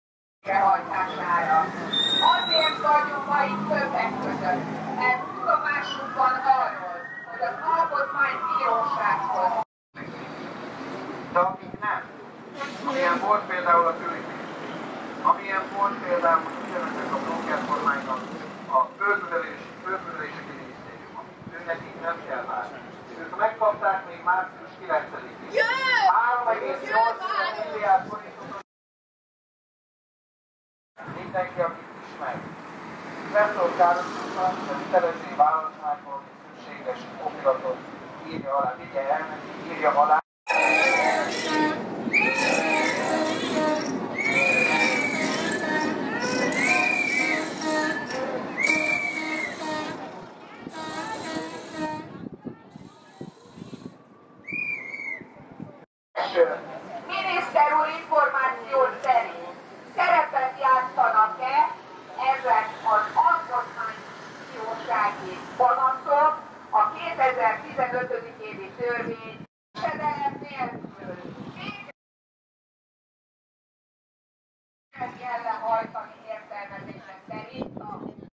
DEMONSTRATION VED UNGARN PARLAMENT 31.3.2016
Denne video handler om BUDAPEST BIKE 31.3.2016
Ikke mange deltagere. Folk tør  ikke give deres mening til kende i dagens Ungarn.